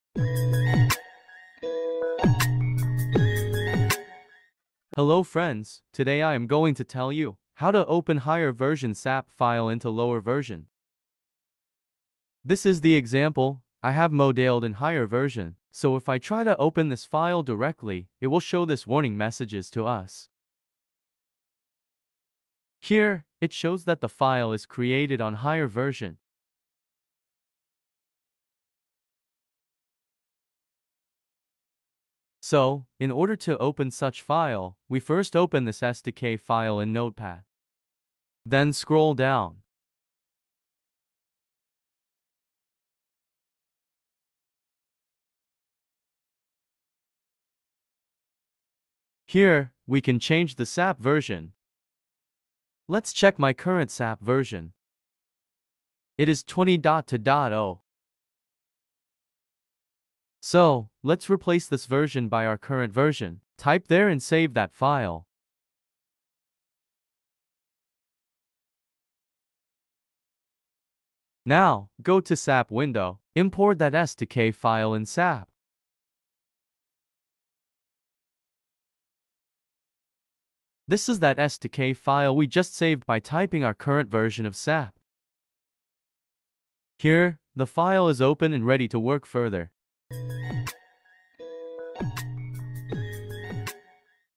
How to open Higher Version SAP file into Lower Version, Importing SAP 2000 File, Quick and Easy Way.
In this video I am going to show, how can we open higher version SAP 2000 model into the Lower Version SAP Program.